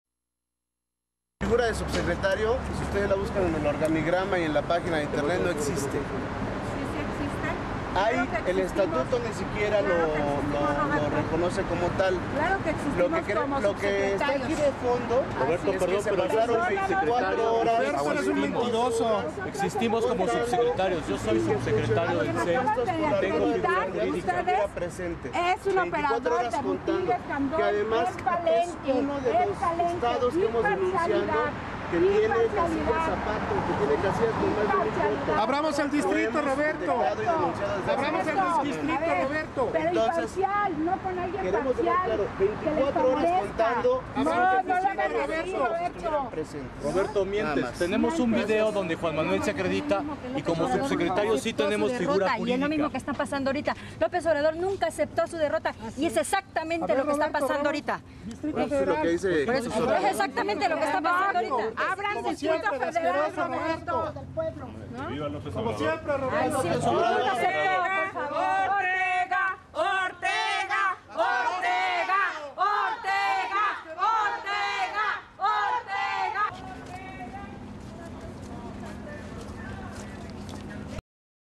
INCONGRUENCIA Y TRAICIÓN DE LOS CHUCHOS
Como pueden ver, para los chuchos no hay congruencia ni hay principios. Todo es la ambición desmedida inclusive a costa de destruir a la izquierda. ESO es Nueva Izquierda: lo que JAMÁS se debe volver a repetir en el PRD.